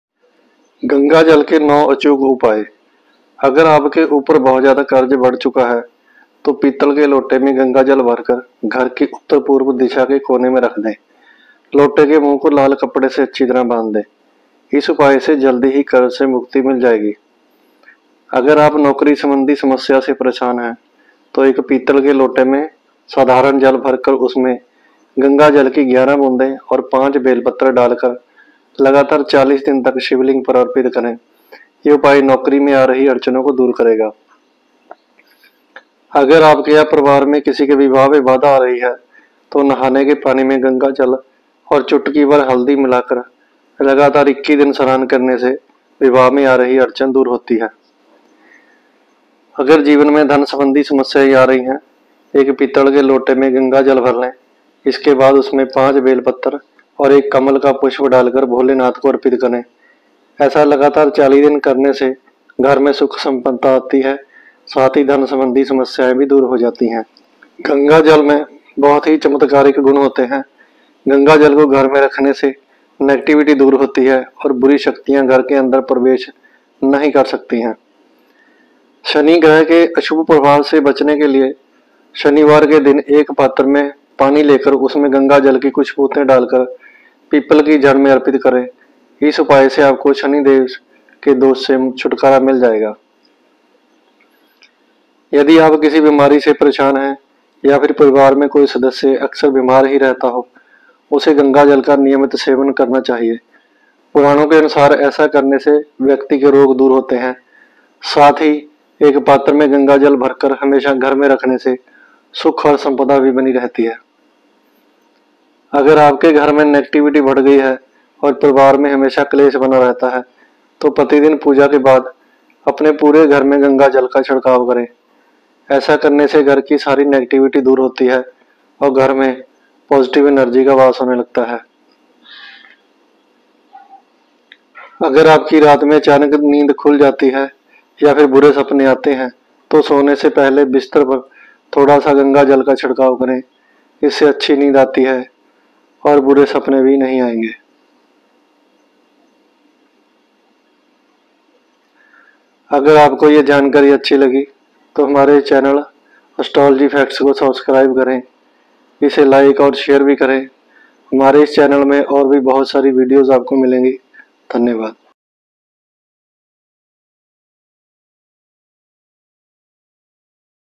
गंगाजल के 9 अचूक उपाय | गंगाजल के ये उपाय बदल देंगे आपकी क‍िस्‍मत | Gangajal ke upaye
gangajal ke upaye
gangajal ke fayde
astrology facts
astrology
vastu tips
vastu shastra